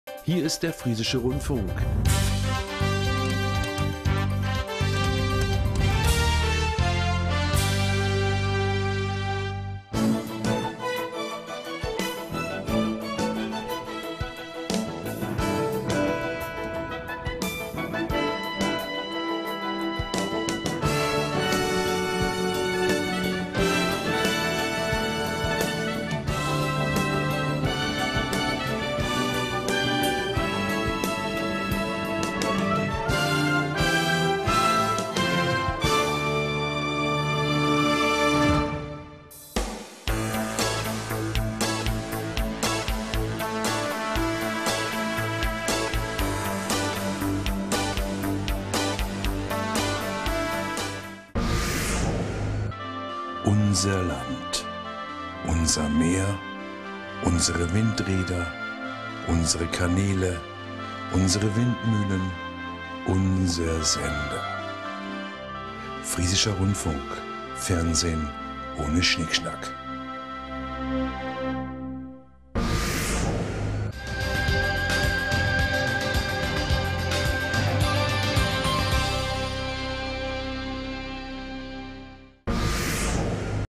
16 Jahre FRF: Der erste Vorspann vom Friesischen Rundfunk
Am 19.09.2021 feierte der Friesische Rundfunk seinen 16. Geburtstag. Dies ist der erste Vorspann, der 2005 erstmals „on Air“ ging.